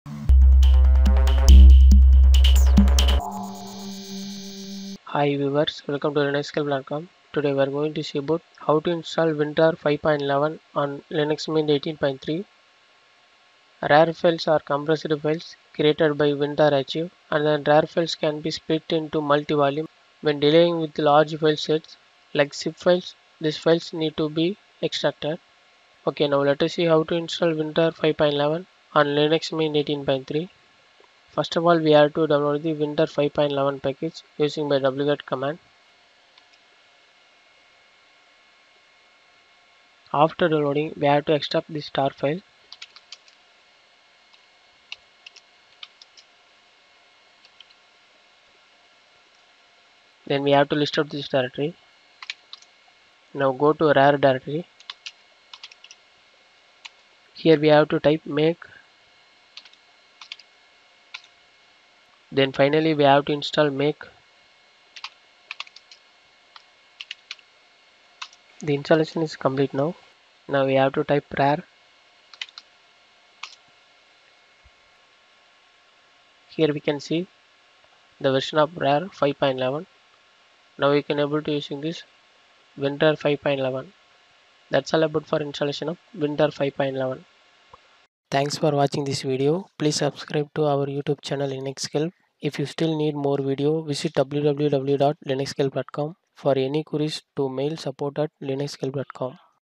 How to install WinRAR 5.11 on Linux Mint 18.3
This video covers the method to install WinRAR 5.11 on Linux Mint 18.3. RAR files are compressed files created by the WinRAR archive.